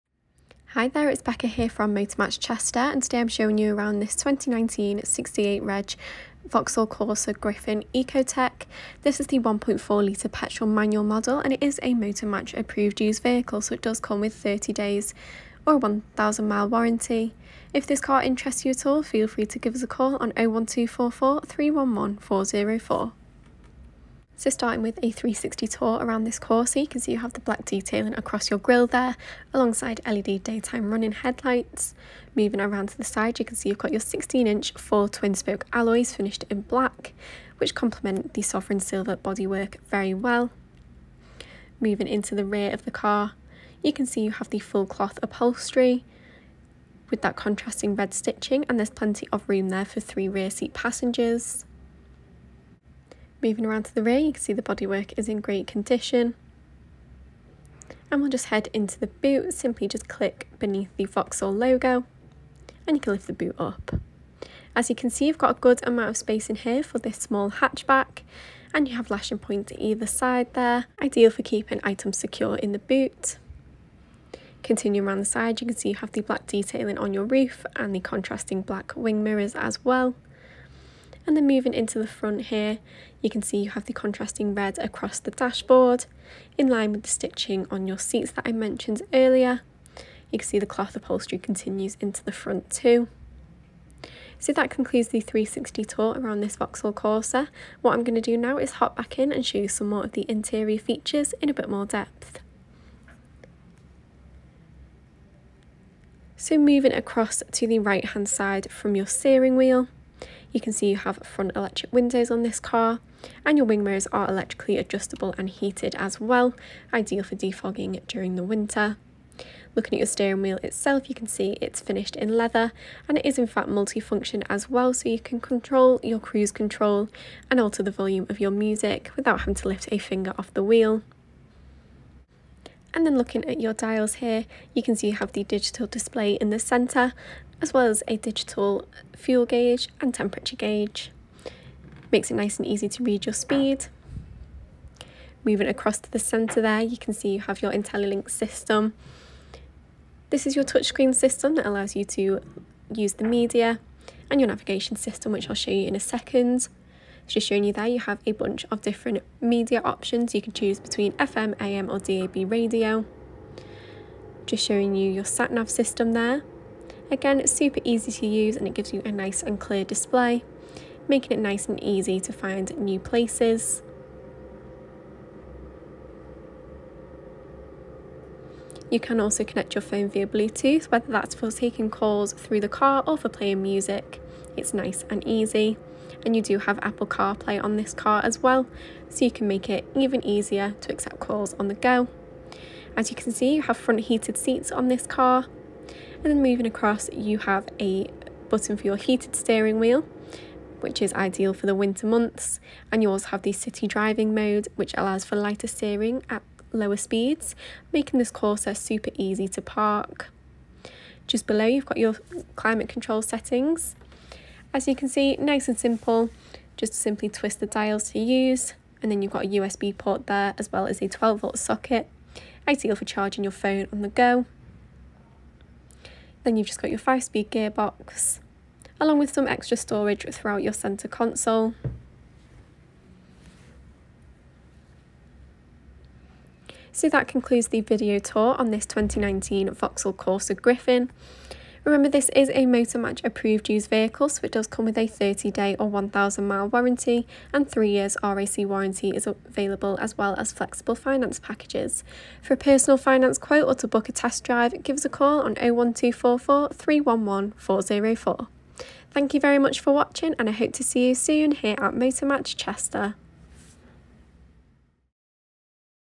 Used 2019/68 Vauxhall Corsa 1.4i ecoTEC Griffin at Chester | Motor Match Used Cars for Sale
Just into our stock, this 2019/68 Vauxhall Corsa 1.4i ecoTEC Griffin in Sovereign Silver, is now available for sale from Chester.

Reg number: DC68 FBZ
Fuel: 1.4L Petrol
Transmission: Manual
Mileage: 8864 miles